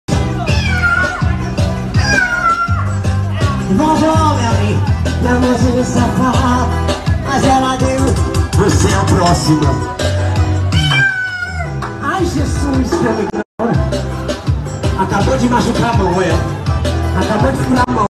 O motoqueiro tá com a mão doendo até agora
Singing and dancing in the middle of the road in traffic